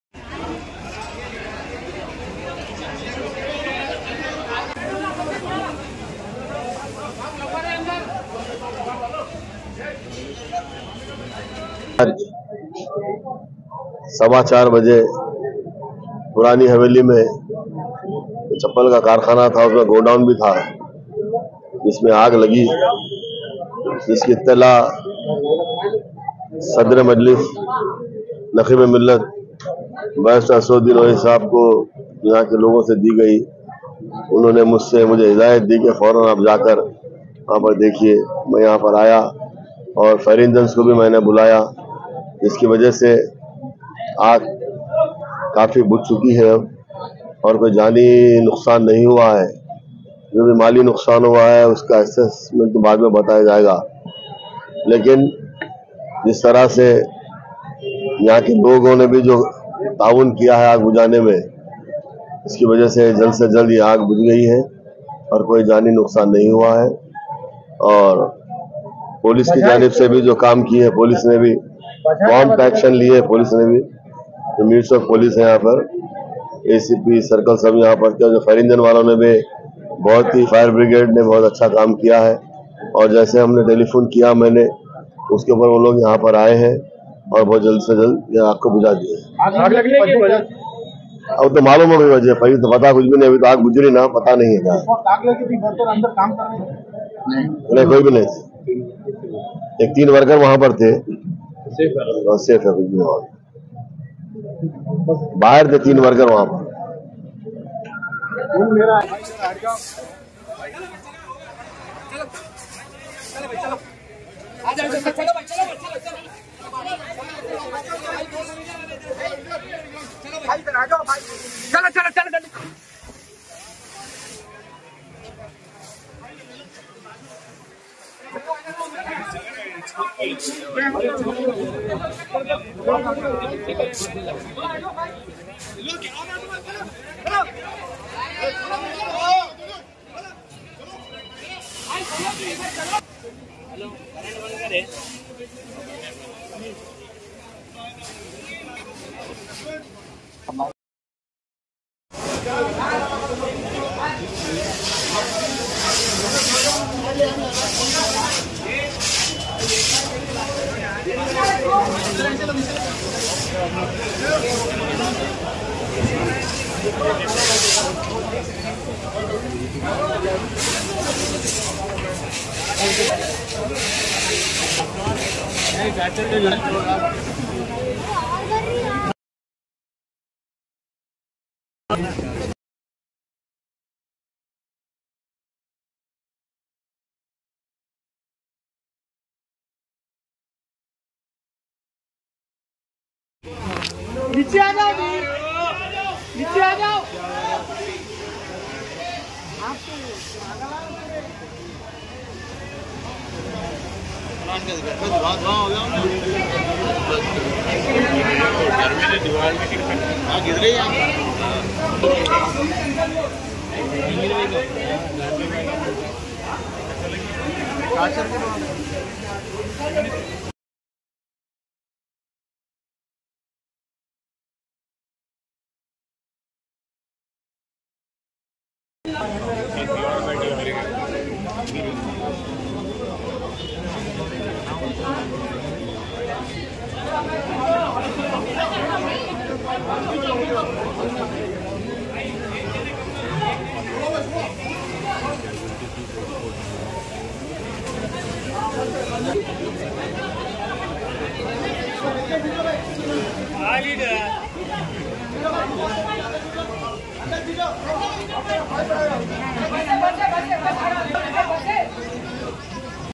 15 Lakh Worth of Spark Rubber Slippers Burnt in Backside Purani Haveli, Fire Due to Short Circuit
15 Lakh Worth of Spark Rubber Slippers Burnt in Backside Purani Haveli, Fire Due to Short Circuit Fire Engine, and Police on Spot.